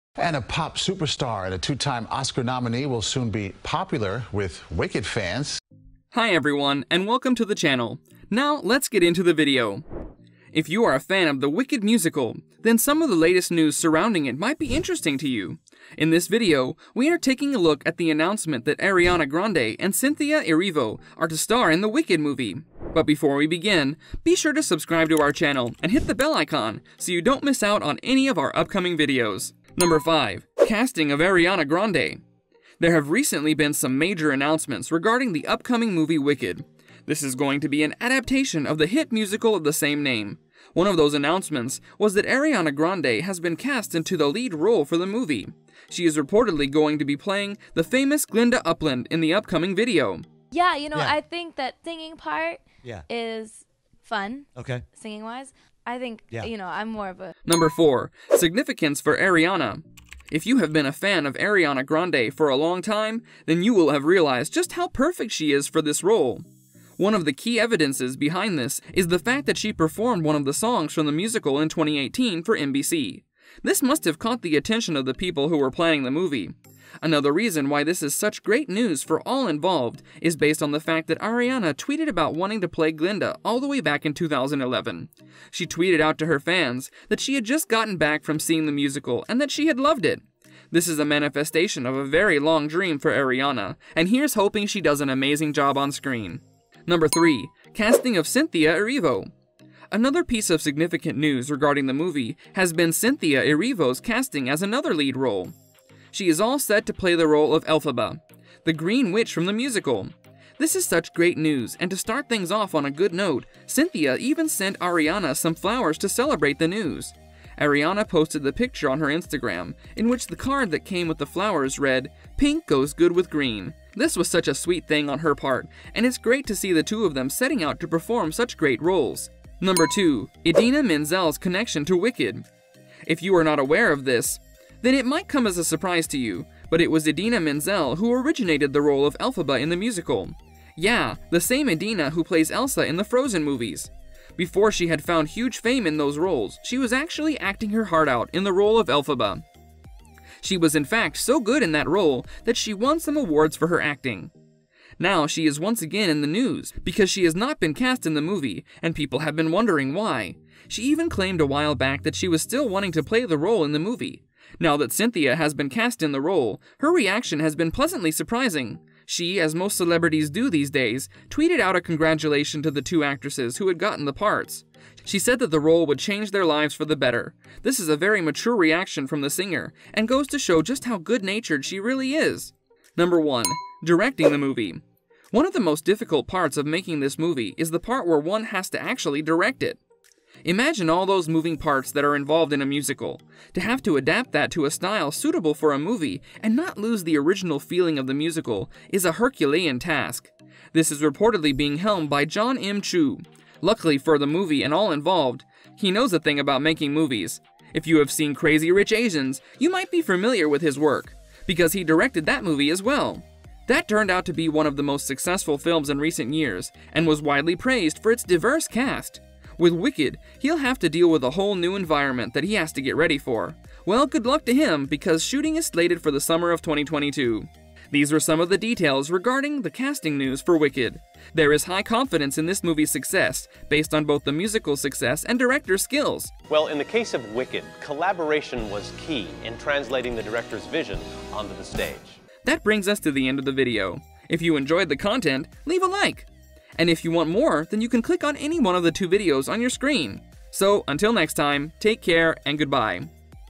Ariana Grande and Cynthia Erivo to Star in 'Wicked' Movie
Hi there, and welcome back to Celebrity Buzz! Ariana Grande and Cynthia Erivo are set to star in Universal's adaptation of the hit musical 'Wicked.' The movie will be directed by Jon M. Chu, who was the director of 'Crazy Rich Asians.' What do you think about the cast?